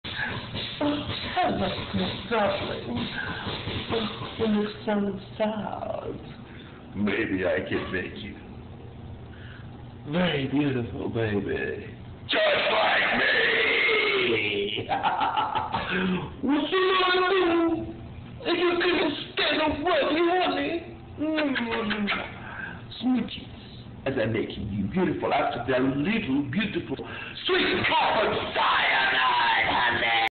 Queen Wendy G Kennedy at The Haunted Mortuary take 1
Queen Wendy G as the Mortician Beutician at The Haunted Mortuary on 4800 Canal St, New Orleans, come and join us and let`s celebrate Halloween at the scariest Haunted House in the Country.